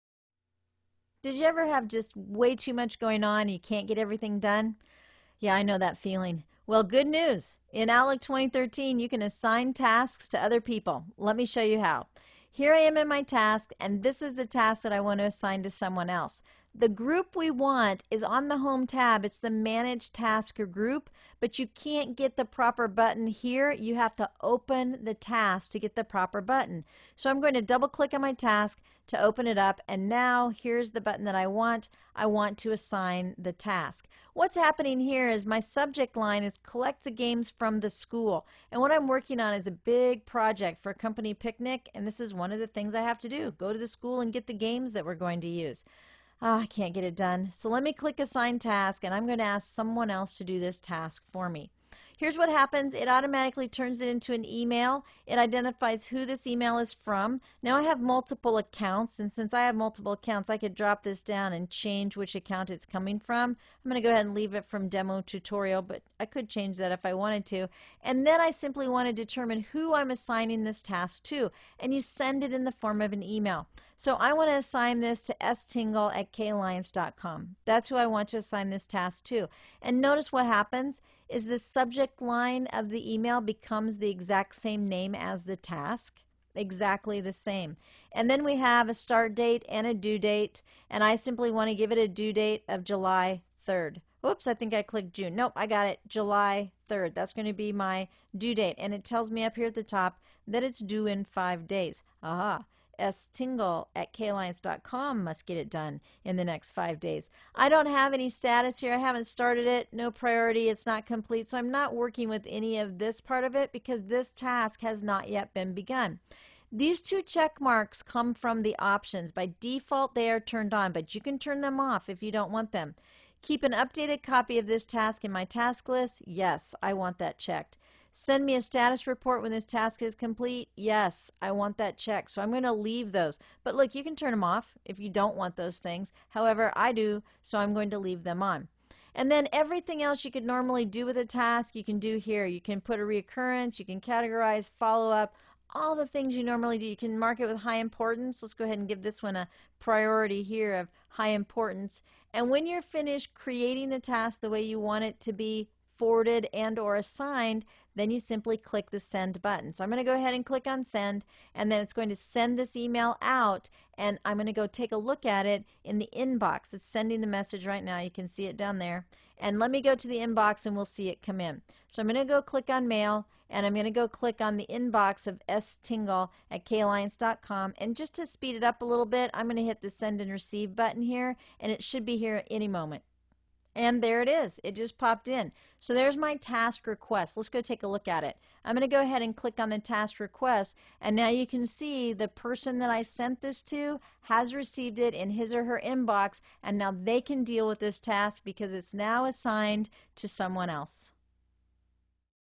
Microsoft Office Outlook 2013: Assigning a Task | K Alliance
Learn how to assign a task in this Microsoft Office Outlook 2013 training video. Contact K Alliance for more info. - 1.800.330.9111